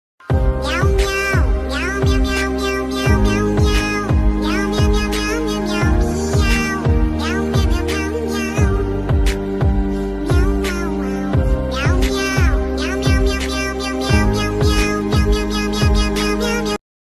Cat Cuts Wrong Side of Hairs & Run From shop
Cat owns a Barber Shop But Cat Cutted Wrong Hair style